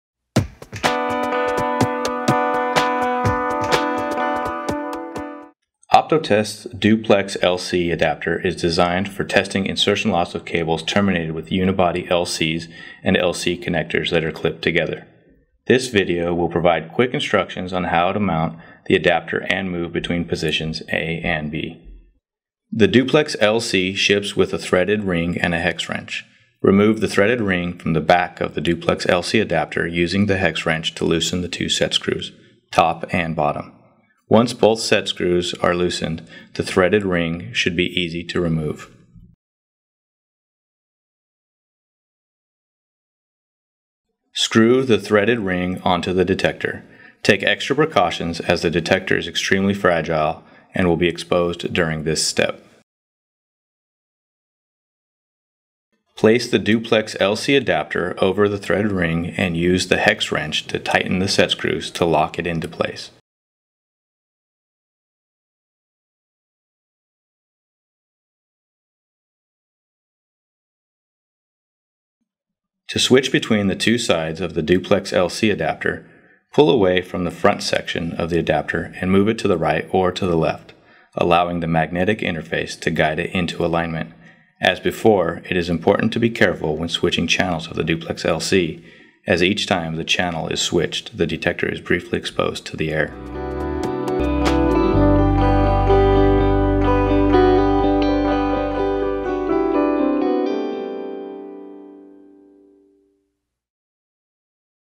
Testing Insertion Loss of cables terminated with Unibody LCs and LC connectors
The Duplex LC adapter is designed for testing insertion loss of cables terminated with Unibody LCs and LC connectors that are clipped together. The adapter screws on to any OptoTest 1mm and 3mm detectors making it ideal for implementing into new or existing test systems. It uses a magnetic interface to allow easy switching between the two LC connectors on a duplex cable.